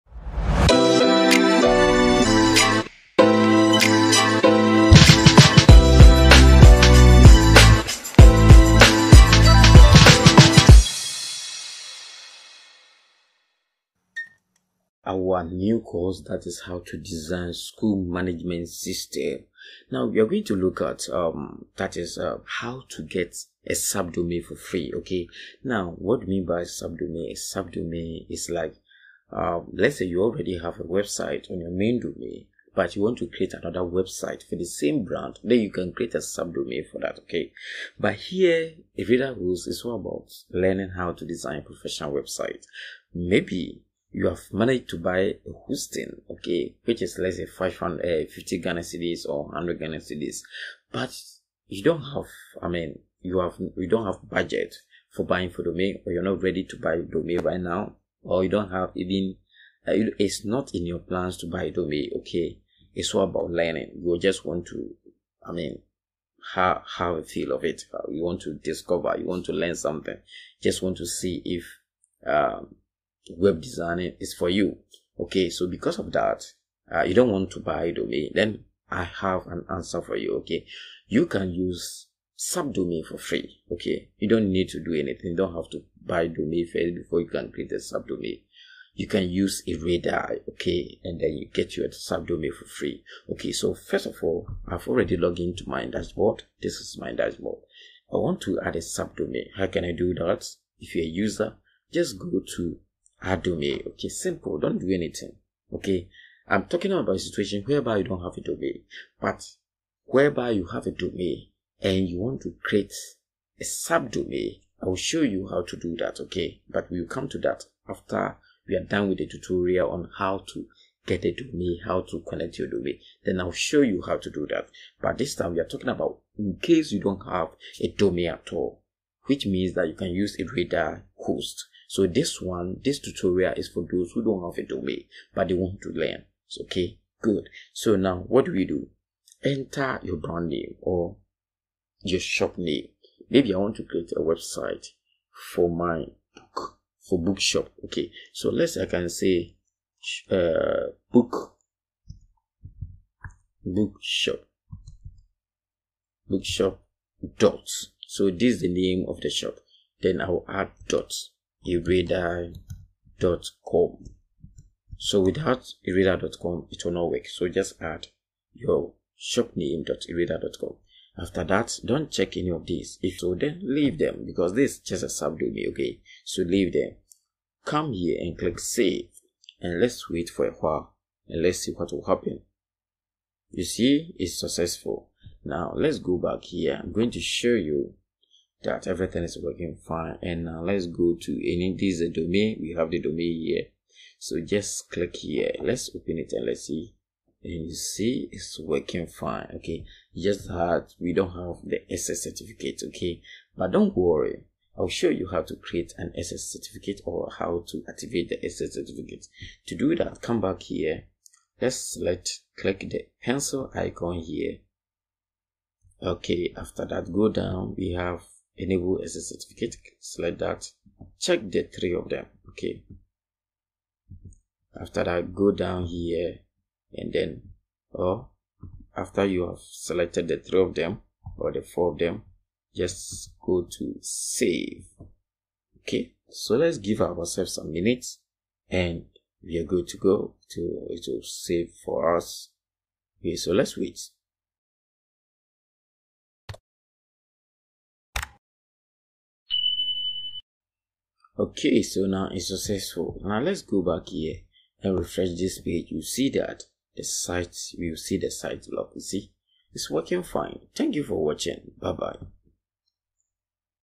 Building a School Management System with WordPress Part 4
Are you a school administrator looking for an efficient way to manage your institution? Look no further! In this video, we will guide you through the process of building a free school management website using WordPress. With WordPress, you can easily create a professional-looking website without any coding skills. Let's get started!
The first step is to install WordPress on your server. This process is simple and can be done in just a few clicks. Once you have WordPress set up, you can begin customizing your website to suit your school's needs.
Next, choose a theme for your website. You can browse through the wide variety of free themes available in the WordPress theme directory. Select a theme that aligns with your school's branding and design preferences.
Now it's time to add functionality to your website. WordPress offers a range of free plugins that can enhance your school management system. Install plugins for features like student registration, attendance tracking, and grade management.
To ensure a seamless user experience, it is crucial to organize your website's content. You can also add a blog section to share school news and updates.
Finally, don't forget to optimize your website for search engines. Install an SEO plugin that will help you improve your website's visibility on search engine result pages. This will make it easier for parents and students to find your school online.
Congratulations! You have successfully built a free school management website with WordPress. Now your school can benefit from a robust online platform to manage student information, communicate with parents, and showcase your institution's achievements.